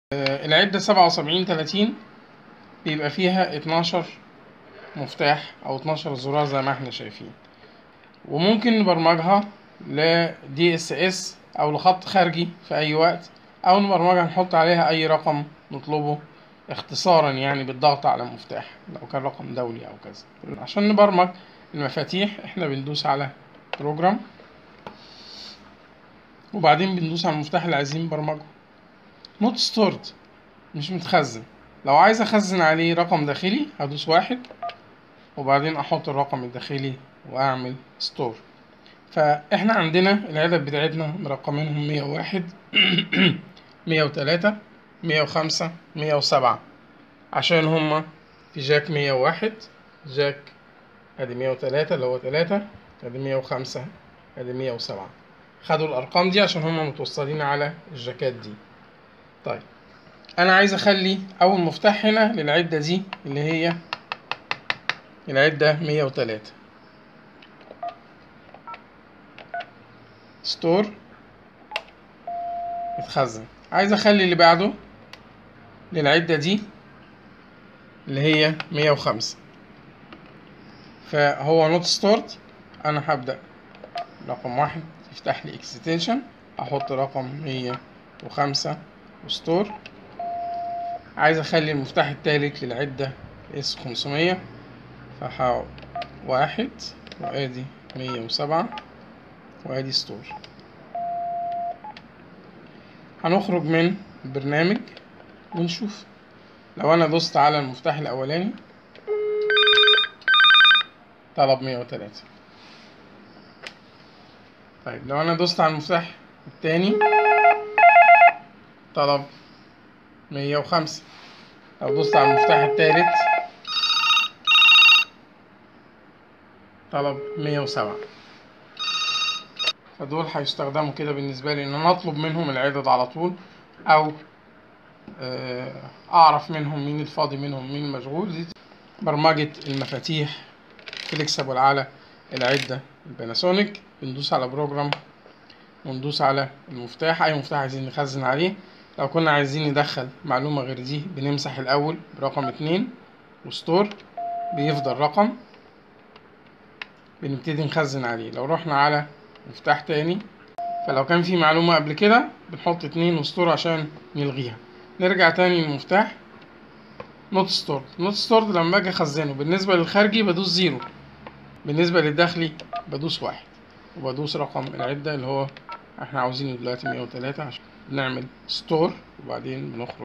تخصيص مفاتيح العده المميزه للطلب الداخلي والخارجي على السنترال باناسونيك - KX-TES 824
-Assigning the special tool keys for internal and external dialing on the Panasonic switchboard
-تخصيص مفاتيح العده المميزه للطلب الداخلي والخارجي على السنترال باناسونيك